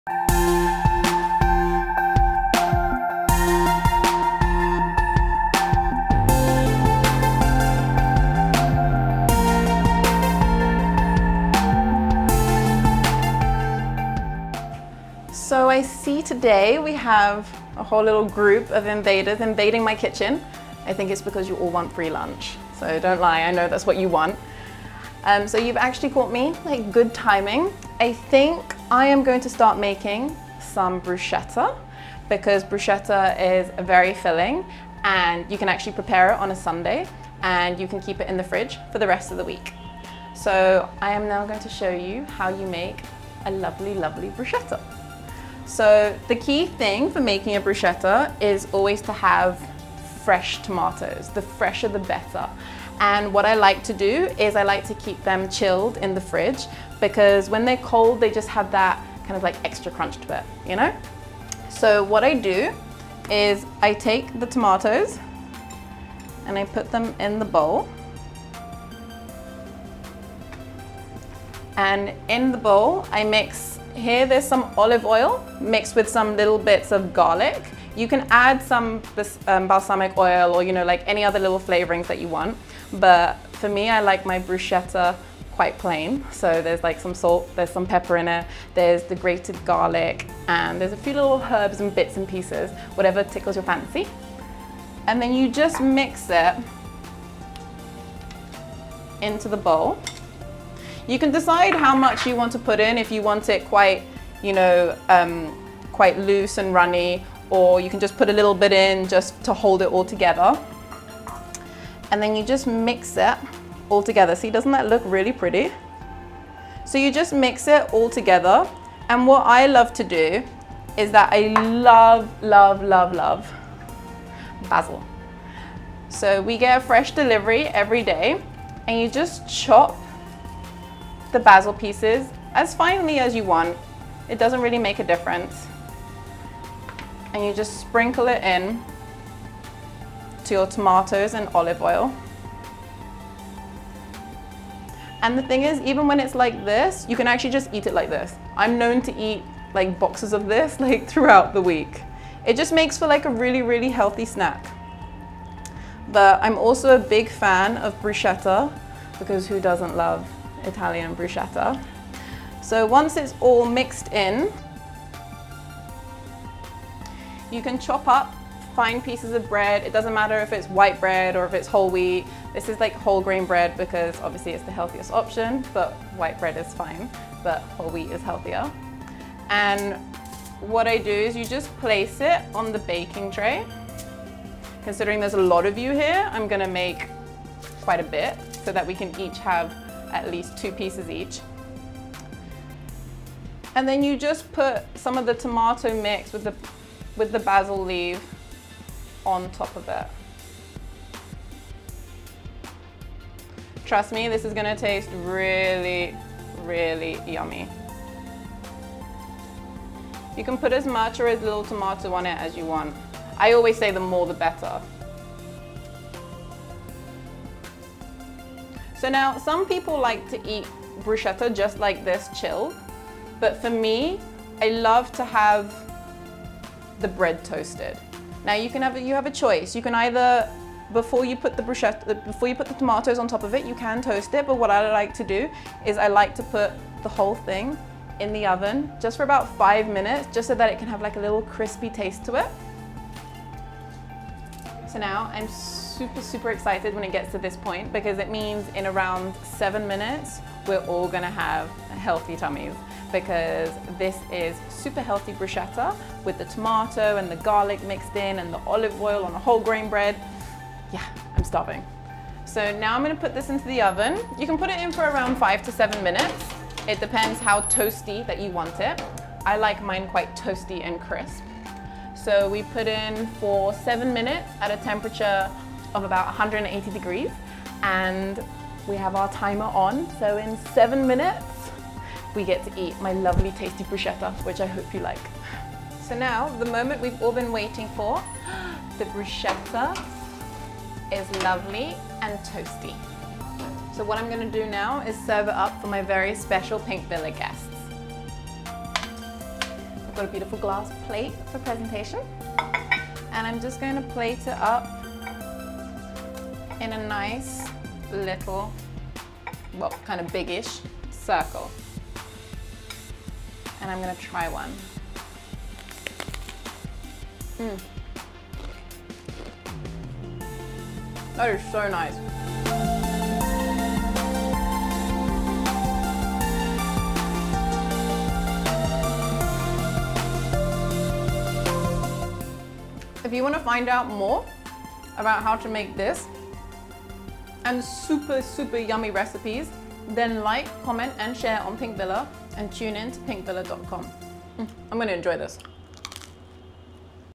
Cook it up with supermodel Elena Fernandes | Healthy vegetarian Bruschetta| In the kitchen | S01E03
Ever wondered how supermodels get their fabulous bodies and manage to look good at all times. Well, we decided to investigate what is cooking in the kitchen of supermodel Elena Fernandes and she very graciously showed us one of her yummy healthy recipes and she hosted Pinkvilla for lunch.

Elena Fernandes cooked it up and made us a Healthy vegetarian Bruschetta. This delicious meal will leave your mouth watering. Watch on to see this super model's vegetarian Bruschetta. SO the next time you are in a fix and want to cook something up quickly this could be your go-to recipe.

You may know her as the girl who played Fawad Khan's agent in the movie Kapoor and Sons. Elena Fernandes is an ace model who is the face of many top brands in India as well as abroad. Born and raised in London, Elena has brands like Adidas, Anita Dongre, Marks and Spencer and Westside in her kitty.